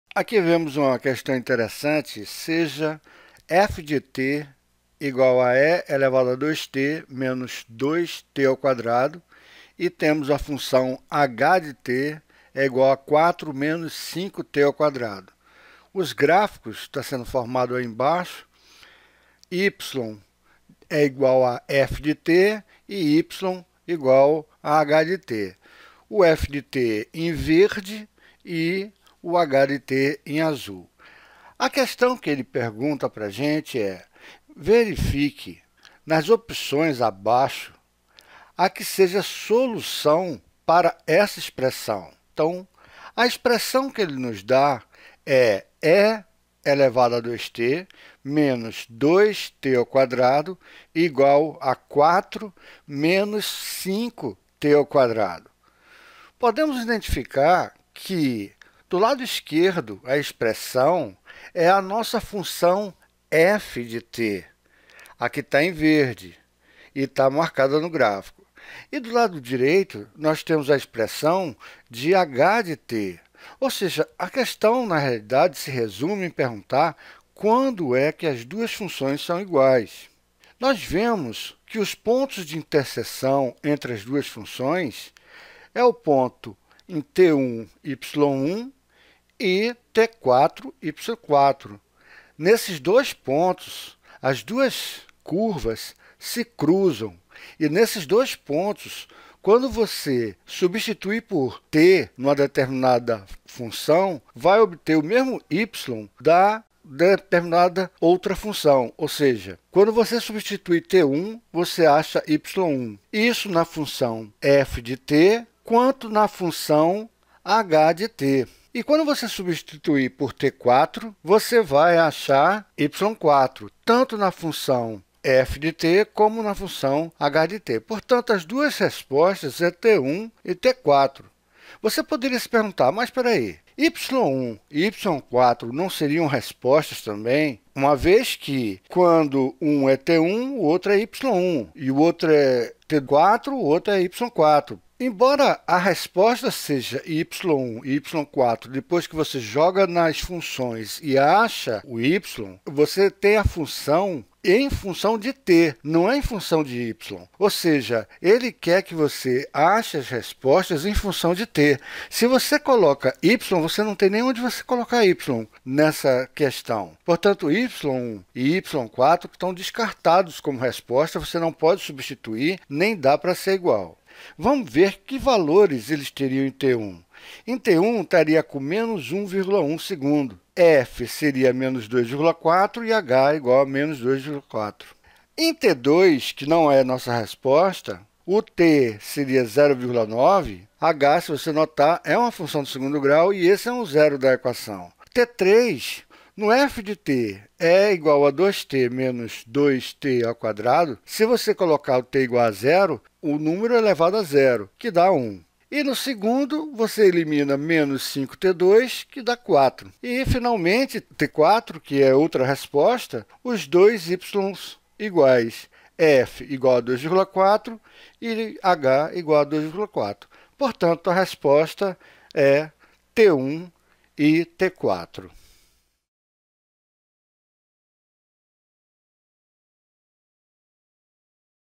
Interpretação de equações por meio de representação gráfica (exemplo 2) | Matemática | Khan Academy
Este vídeo ensina: Sal encontra as coordenadas dos pontos de um gráfico que são soluções para determinada equação.

Perdeu a aula anterior?

Equações avançadas e funções na Khan Academy: Análise feita através de duas funções, que ao serem igualadas, chega-se conjunto solução ao verificar as interseções dos respectivos gráficos.

Quer um tutor virtual para Equações avançadas e funções?  Habilite a missão Equações avançadas e funções na Khan Academy